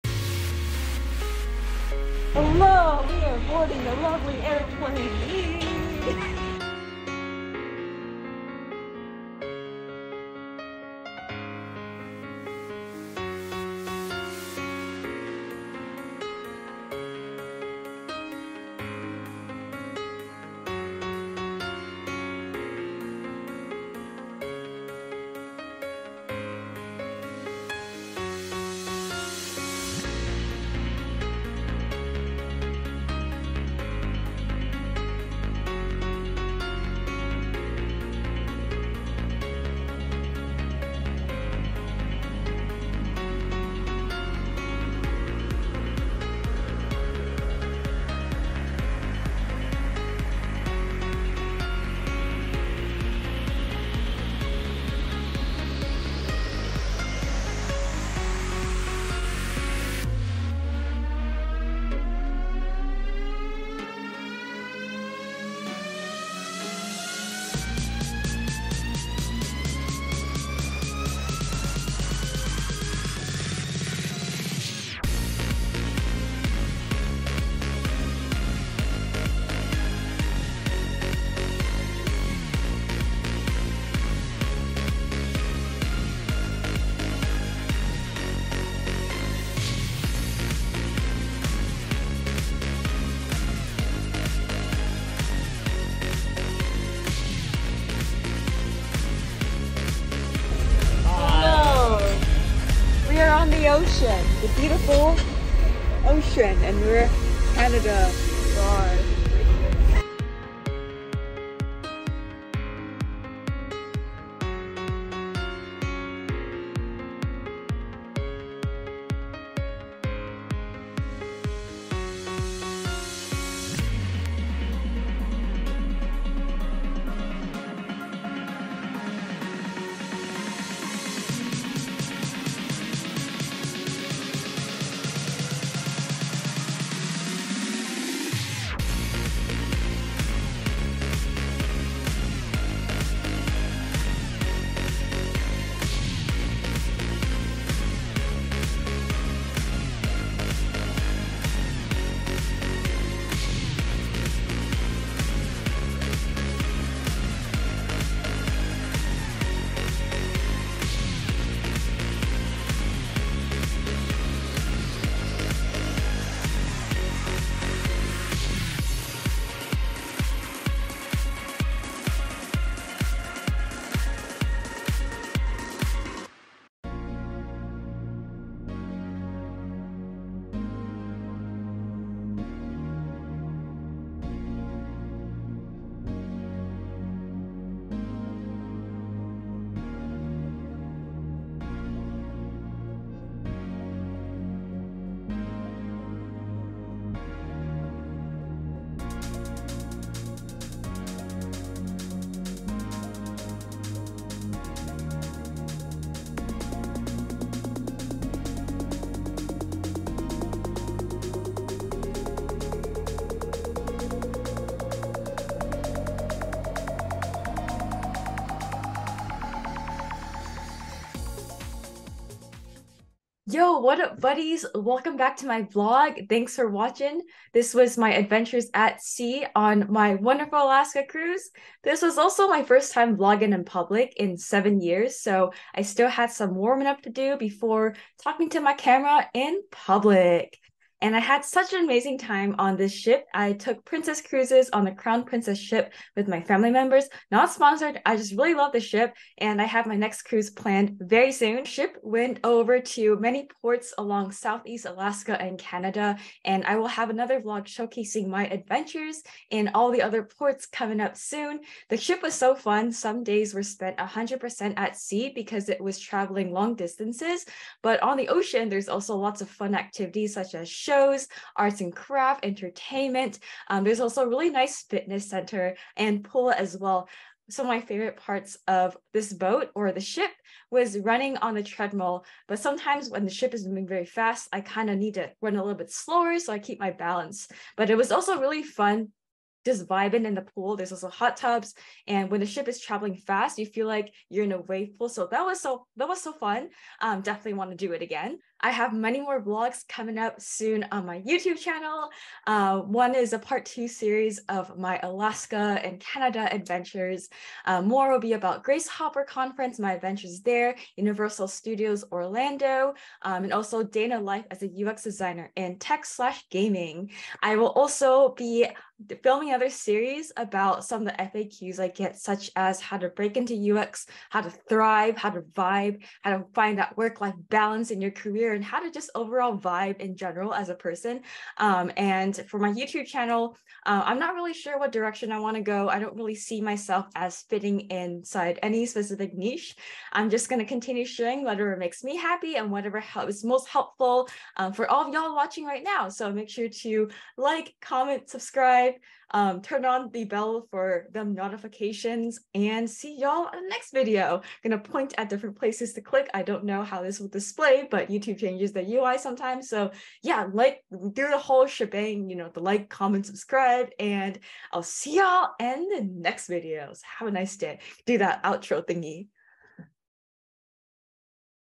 Alaska Cruise - Life at sea 🌊
Showcasing some highlights & takeaways of life at sea on the cruise! It was my first cruise - I had an amazing time and have my next one planned. I boarded the Crowne Princess ship with my family.

Patrick Patrikios